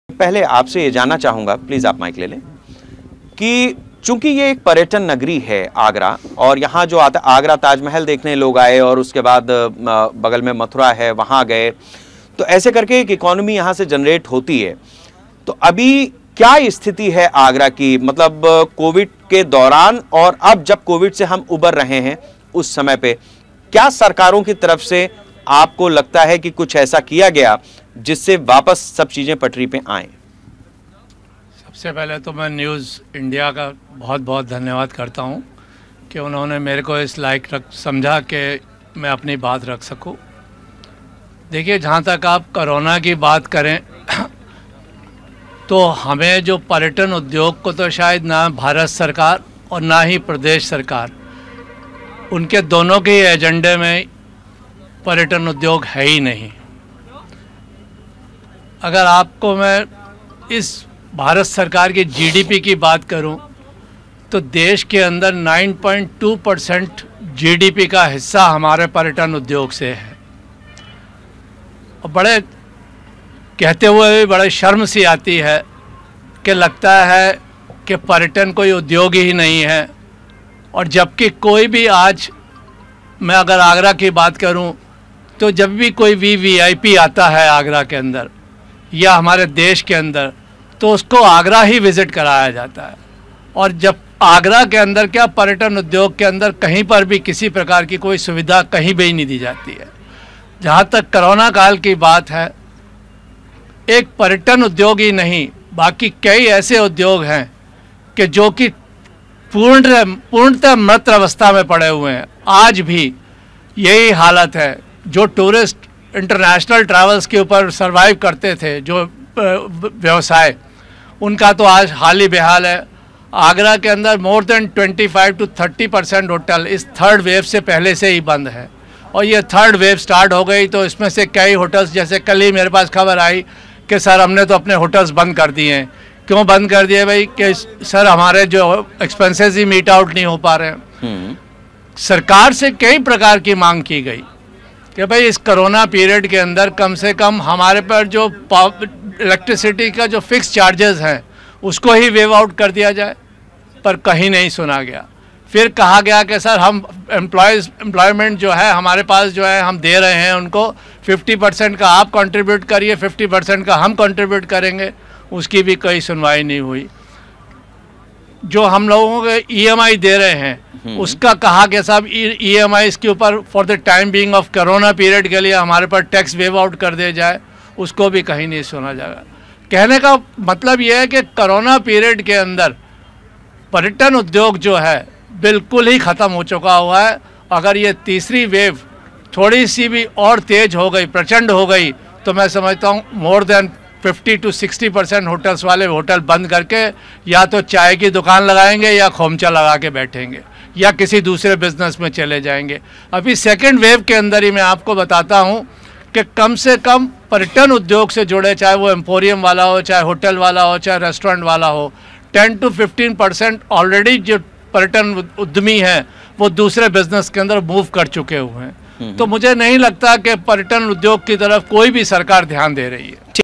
Agra Conclave On News India : आगरा में न्यूज़ इंडिया का सियासी मंच
Agra Conclave में समझिए यूपी का गणित , ताज नगरी में ताज किसका ?